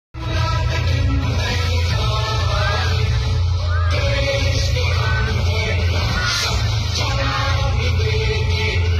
Women Commandos India's first women CoBRA Commandos to be deployed in naxal .
Women Commandos 👍🏻
India's first women CoBRA Commandos to be deployed in naxal affected areas .
(VC DDNewslive )